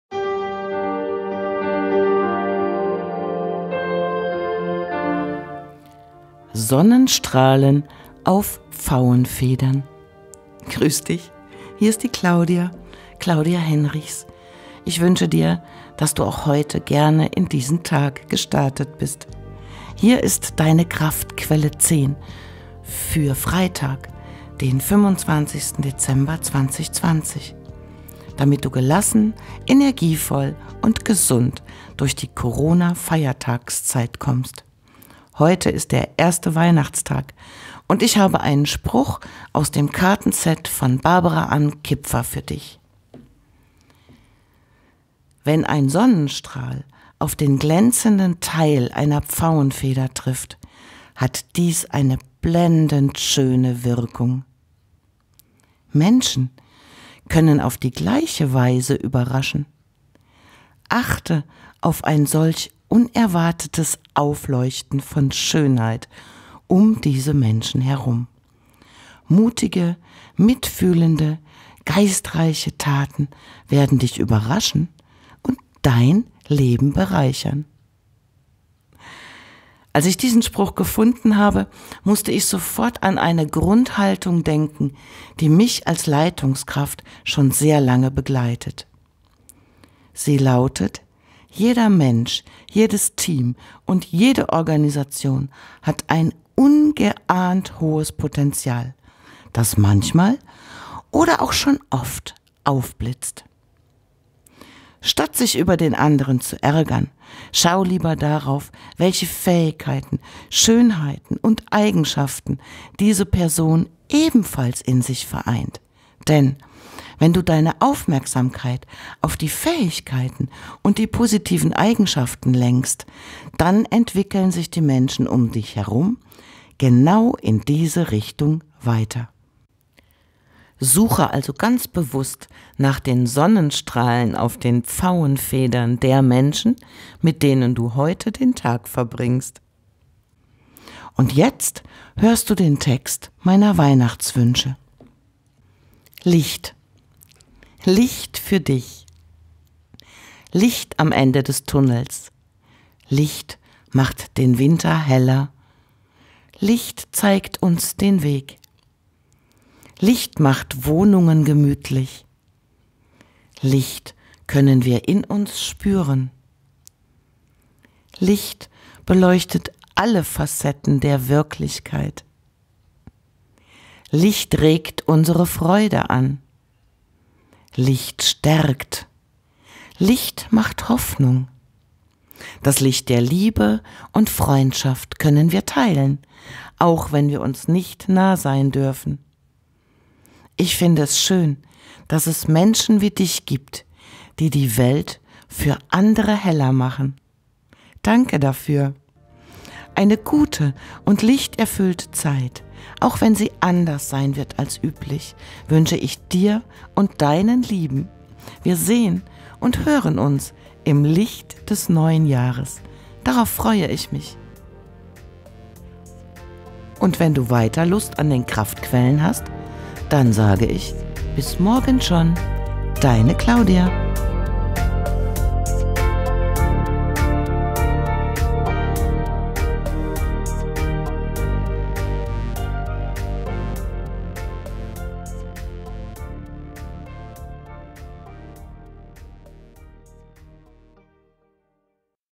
Sonnenstrahlen auf Pfauenfedern
Kraftquelle 10 für den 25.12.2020
Damit Du gelassen, energievoll und gesund durch die Corona-Feiertags-Zeit kommst!
Statt sich über den anderen zu ärgern, schau lieber darauf, welche Fähigkeiten, Schönheiten und Eigenschaften diese Person ebenfalls in sich vereint.
Suche also ganz bewusst nach den Sonnenstrahlen auf den Pfauenfedern der Menschen, mit denen Du heute den Tag verbringst.

Quelle: Achtsamkeit, 52 Karten von Barbara Ann Kipfer